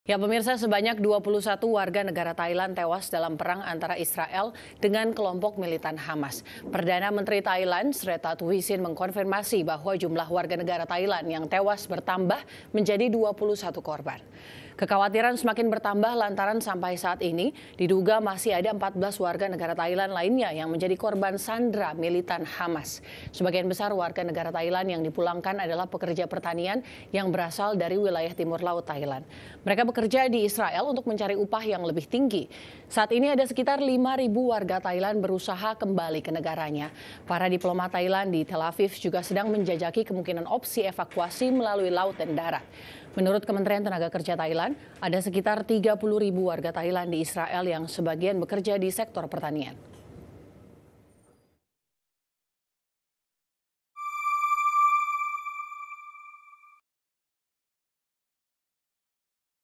21 Warga Thailand Tewas Dalam Perang Israel-Hamas | IDX CHANNEL
Sebanyak 21 warga negara Thailand tewas dalam perang antara Israel dan Hamas. Perdana Menteri Thailand, Srettha Thavisin mengkonfirmasi bahwa, jumlah warga negara Thailand yang tewas bertambah menjadi 21 korban, kekhawatiran semakin bertambah lantaran sampai saat ini diduga masih ada 14 warga negara Thailand lainnya yang menjadi korban sandera militan Hamas.